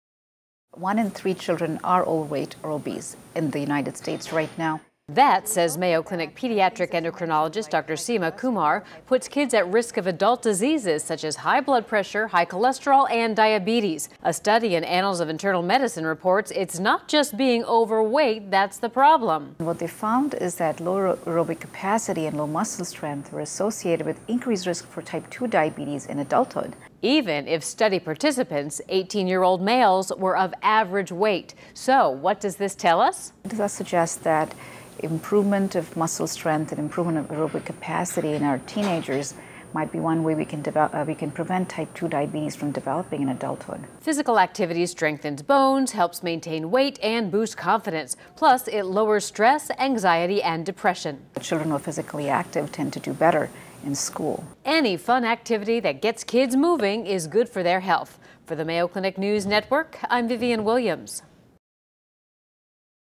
Mayo Clinic Minute: Out of Shape Kids and Diabetes
A new study shows teens who are out of shape are also at increased risk for type-2 diabetes in the future, even if they are not over weight.

Reporter Vivien Williams talks to Mayo Clinic pediatric endocrinologist Dr. Seema Kumar about this issue and ways to prevent it.